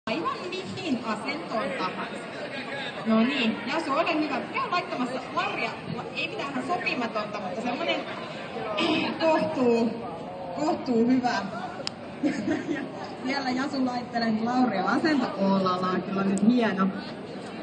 4D SÄÄ 5.9.2014 19:31 ROVANIEMI (3D VIDEO)
+14 ja rupesi vettä ripsomaan kesken Taiteiden Yötä (tai siis iltaa).